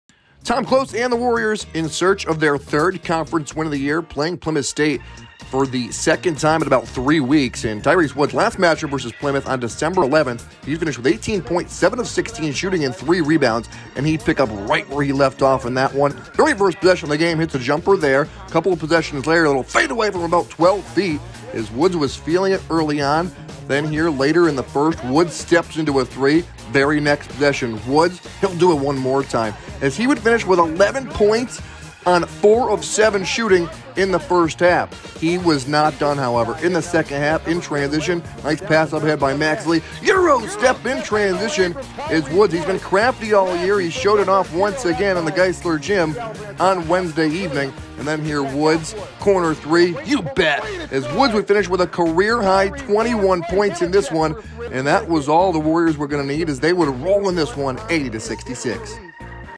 1.5.22 MBB VS. PLYMOUTH ST.
The Warriors would pick up their 7th home win of the year and 3rd conference win as they took down Plymouth St. 80-66 on Wednesday Night thanks to a career night for Tyreice Woods. Check out the highlights!